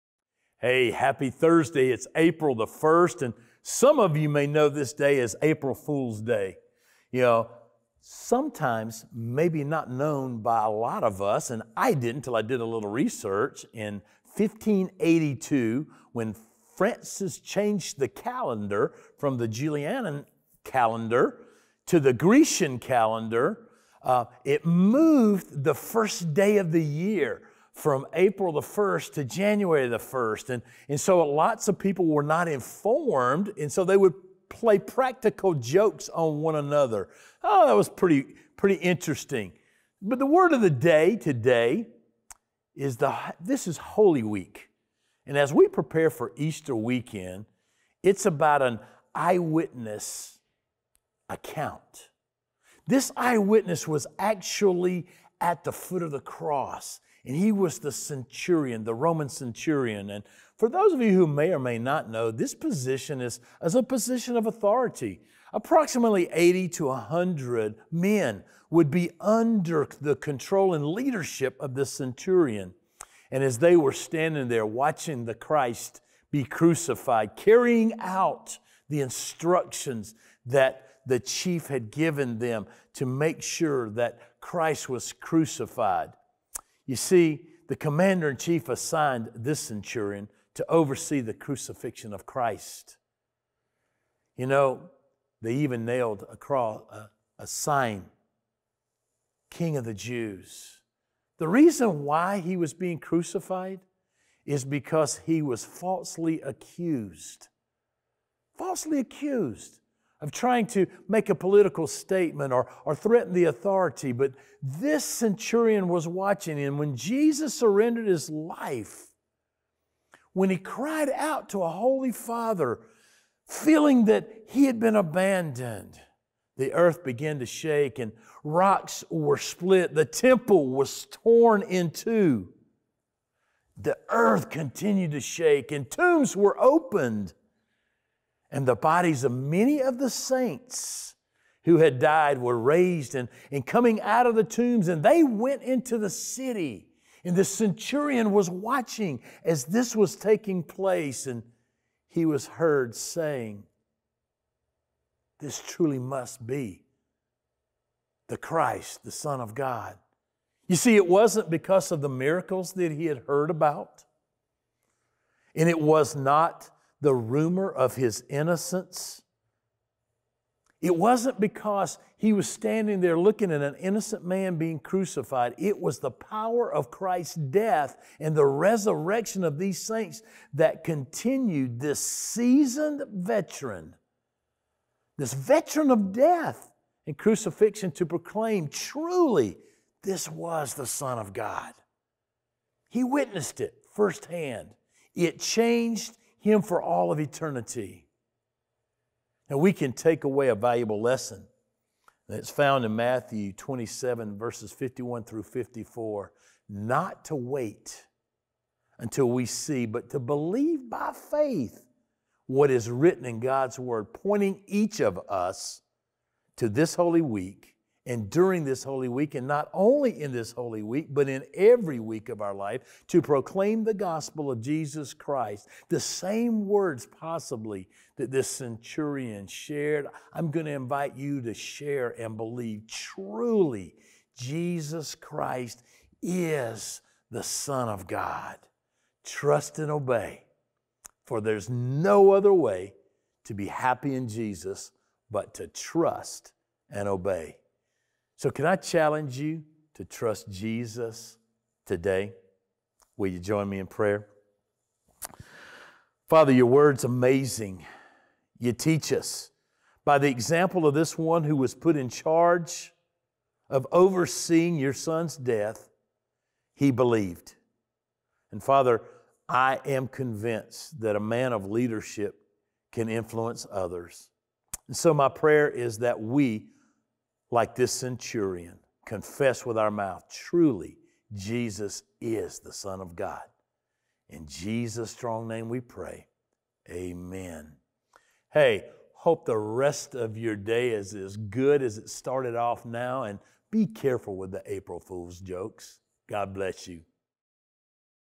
Your Word for the Day - Episode 268
Pastor Chet talks about the Centurion, the Roman soldier who after seeing Christ’s crucification, repented and acknowledged Jesus’s holiness.